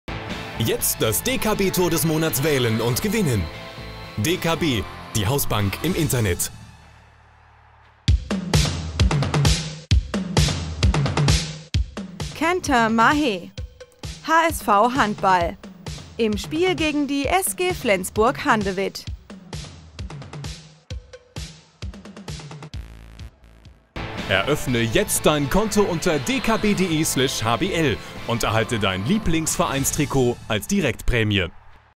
DKB Tor des Monats Dezember - Kentin Mahé - TOR 4
TOR 4: Kentin Mahé, HSV Handball, im Spiel der DKB Handball-Bundesliga gegen SG Flensburg-Handewitt.